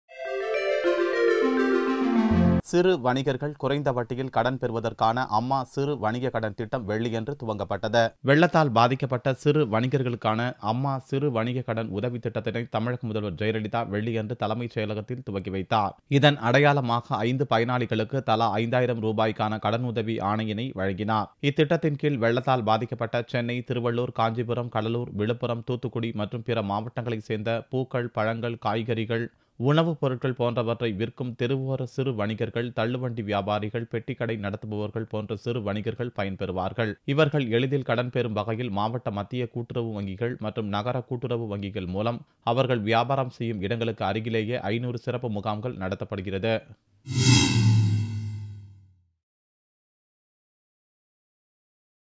TN CM Jayalalithaa inaugurates free loan for flood affected small traders -
Jayalalithaa inaugurates interest free loan for flood affected small traders - Dinamalar Jan 22nd 2016 For more videos